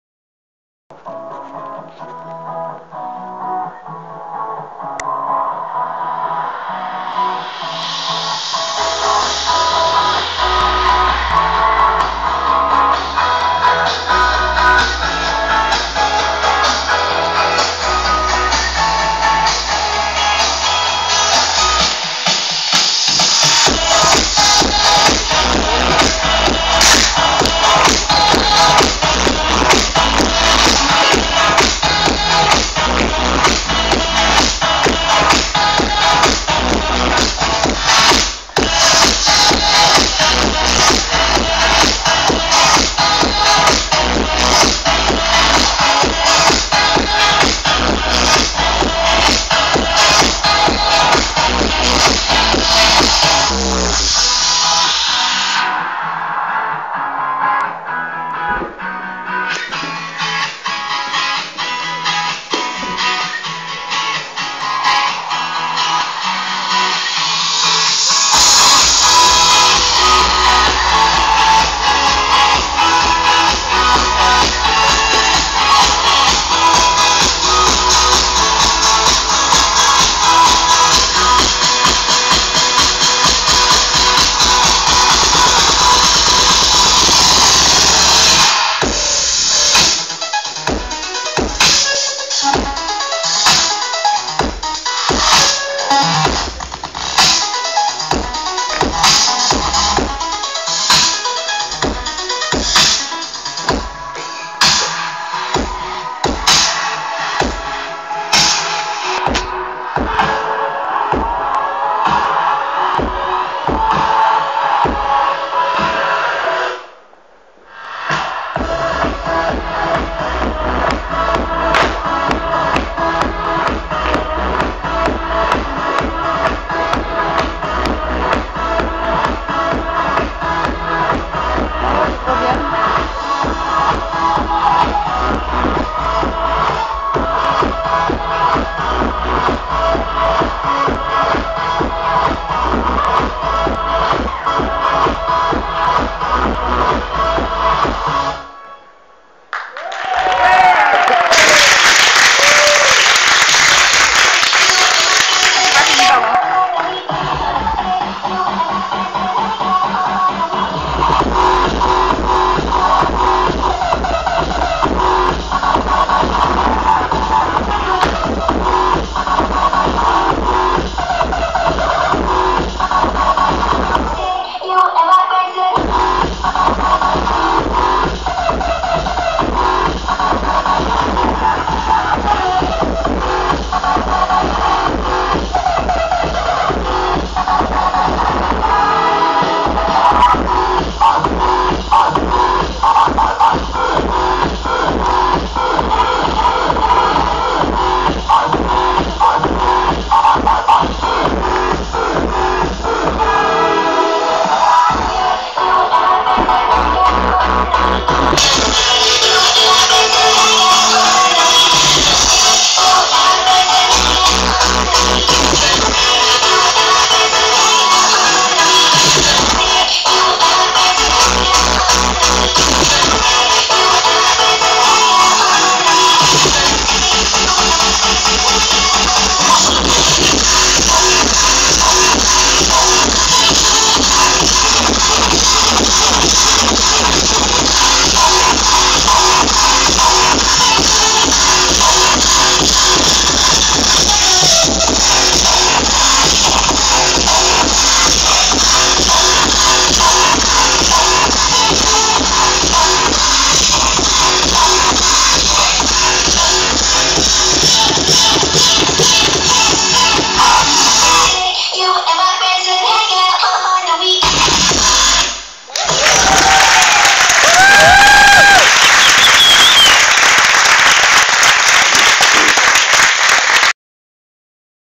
Rentrée NC 1ère - ENDEMIX
A l'occasion de la rentrée de NC 1ère, la chaîne locale a proposé un petit spectacle de danse créé par les jeunes du Rex. Ca bouge, ça change !